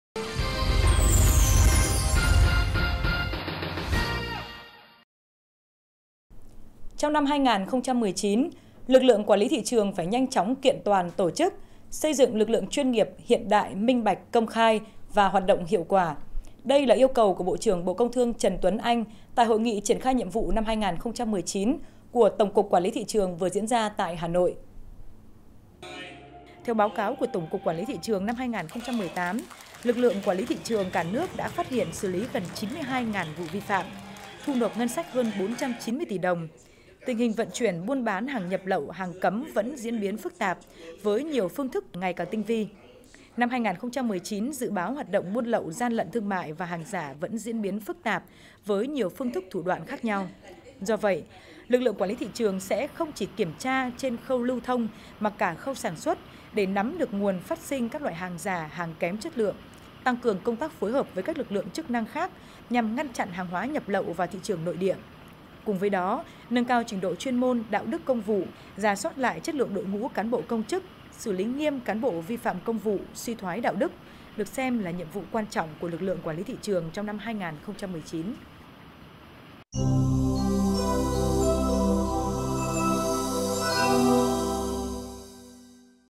Xây dựng lực lượng QLTT: chuyên nghiệp – hiện đại – minh bạch – công khai – hoạt động hiệu quả
Trong năm 2019, lực lượng quản lý thị trường phải nhanh chóng kiện toàn tổ chức, xây dựng lực lượng chuyên nghiệp, hiện đại, minh bạch, công khai và hoạt động hiệu quả. Đây là yêu cầu của Bộ trưởng Bộ Công Thương Trần Tuấn Anh tại Hội nghị triển khai nhiệm vụ 2019 của Tổng Cục quản lý thị trường vừa diễn ra tại Hà Nội…